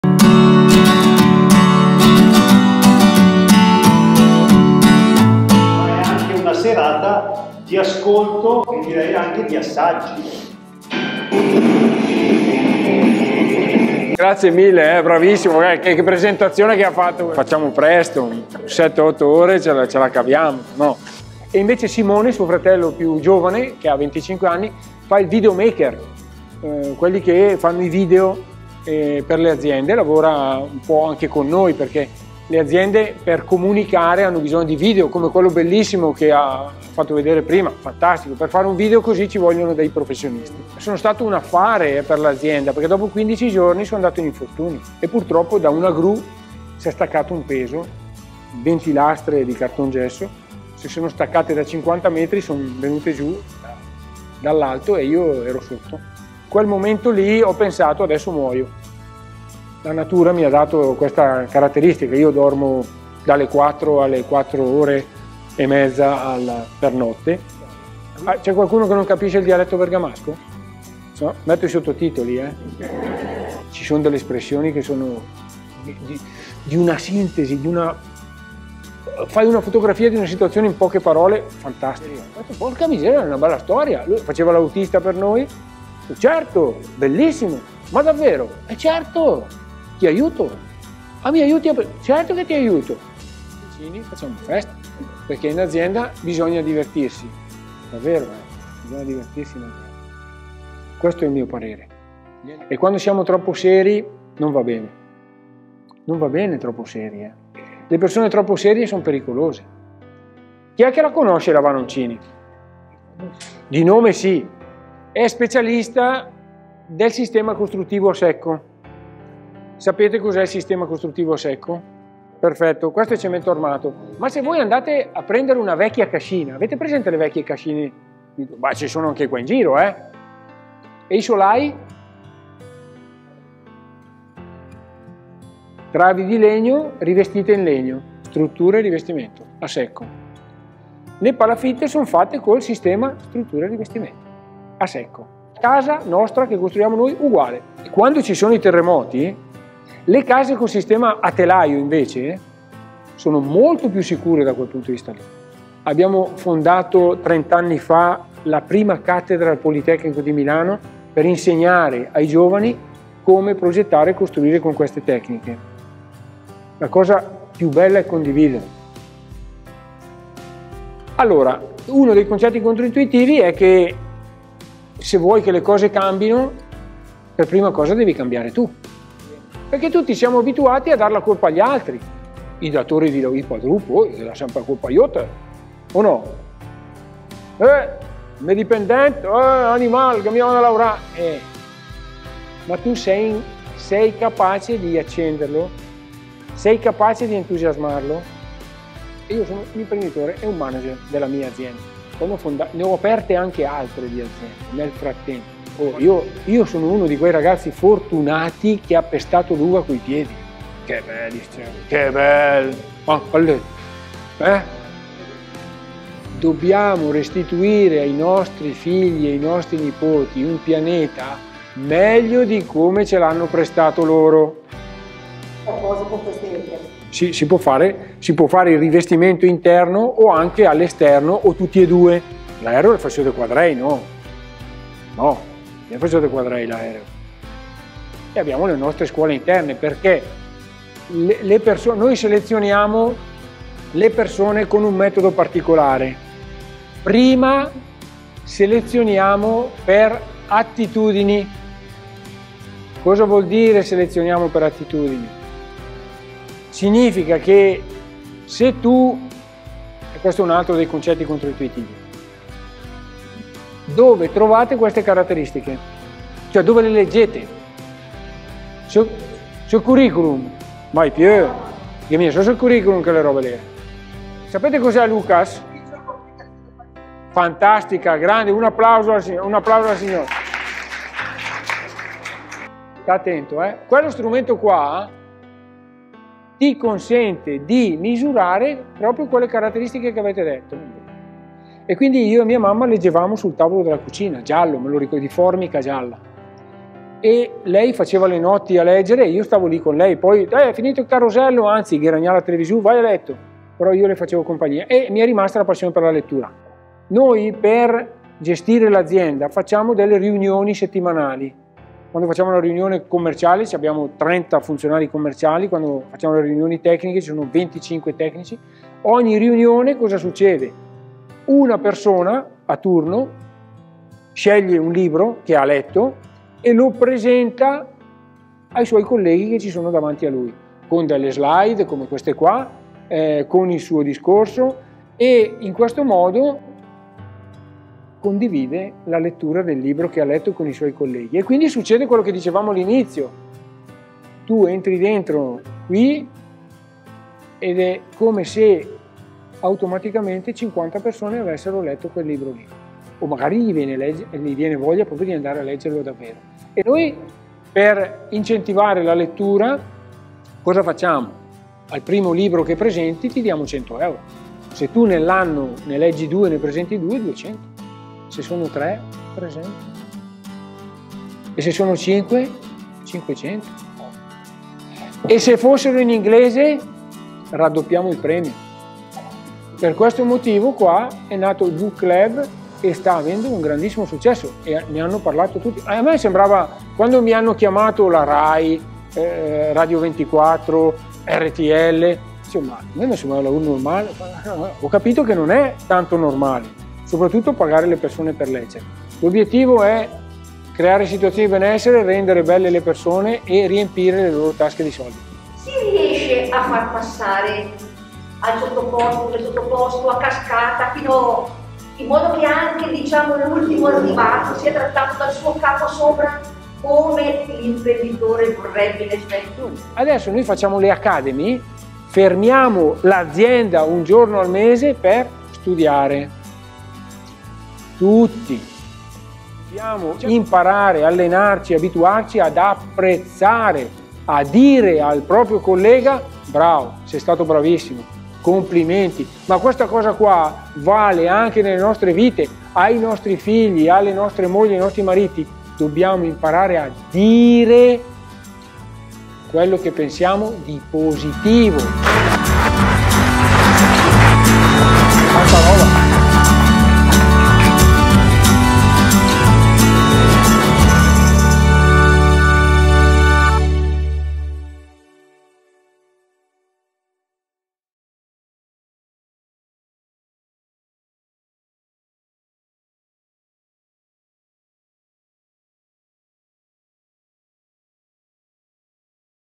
Aperiracconto con Danilo Dadda, A.D. Vanoncini S.p.A ⎜Trailer
23 marzo 2022
ore 19.00 Oratorio S.G.Bosco Seriate
via Mons. Carozzi, 19
Incontri sul Serio - associazione IL GRETO Seriate